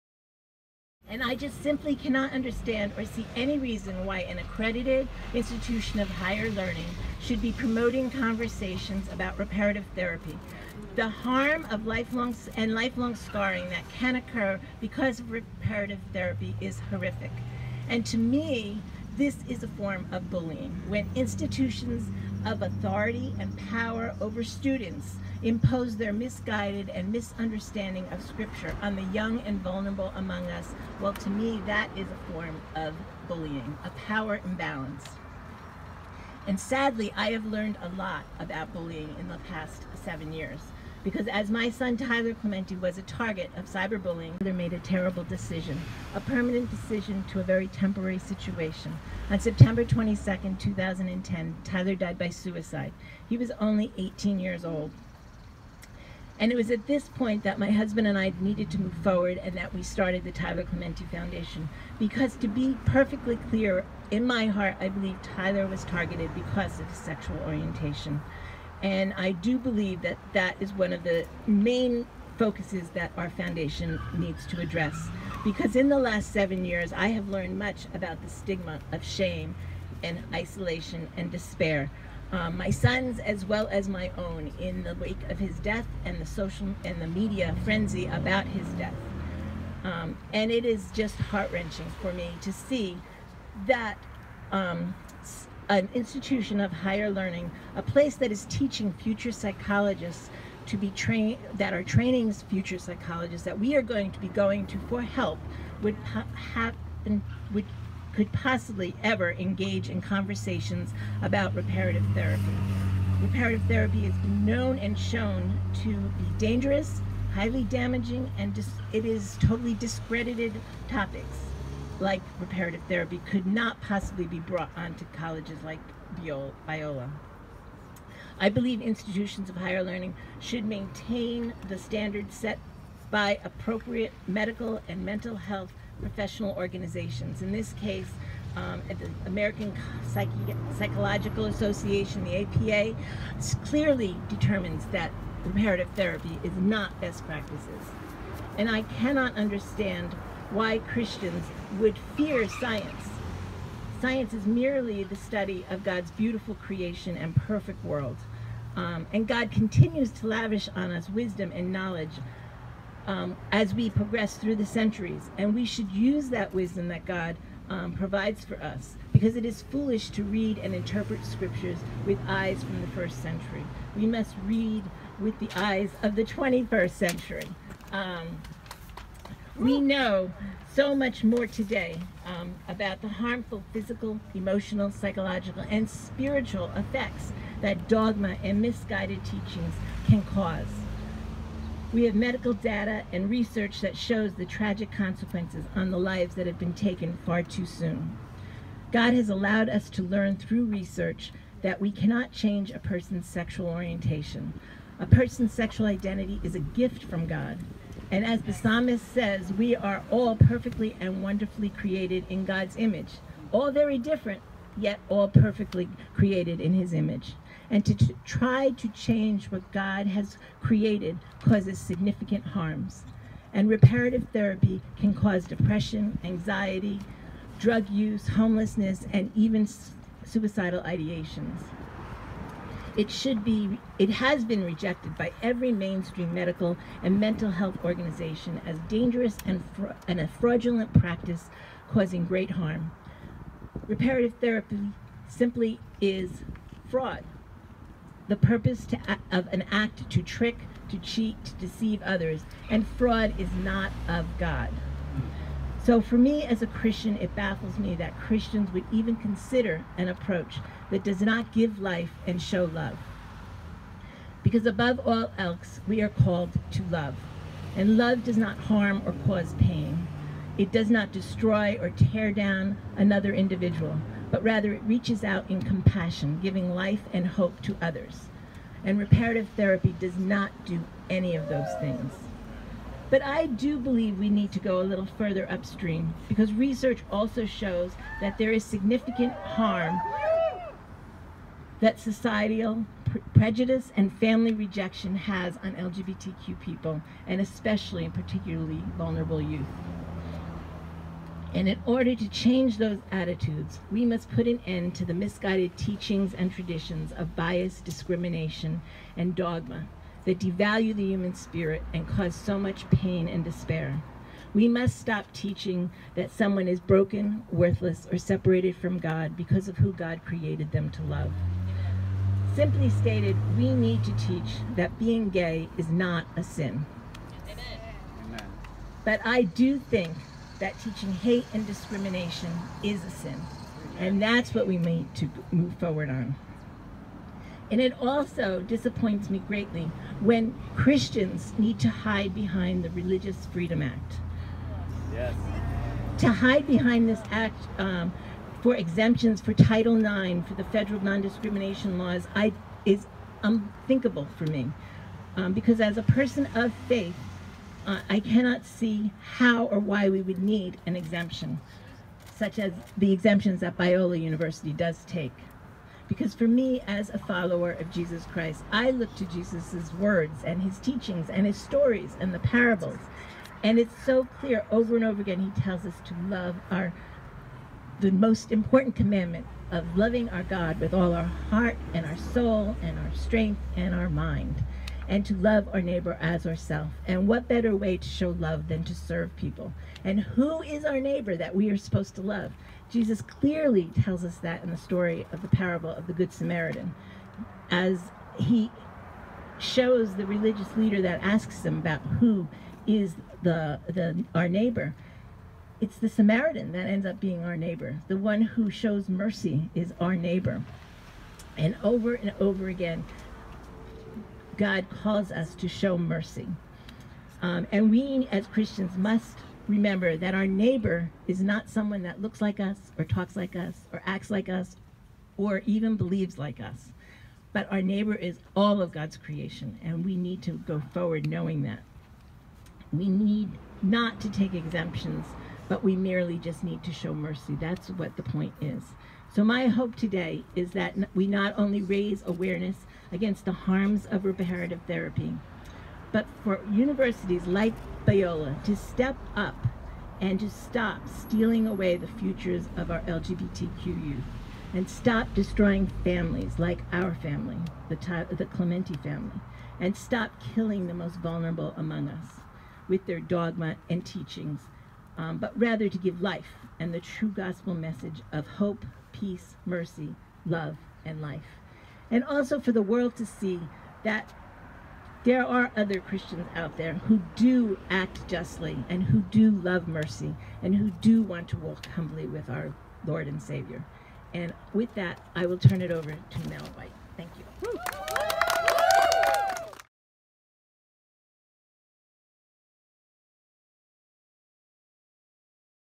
True Faith Doesn't Bully: Jane Clementi Speaks at Biola University Demonstration
On December 4, 2017, the Tyler Clementi Foundation travelled to Biola University to stand up against religious-based bullying. Tyler Clementi Foundation Co-founder and mother of Tyler Clementi shared Tyler's Story experience and her commitment to end the religious-based bullying of LGBTQ people.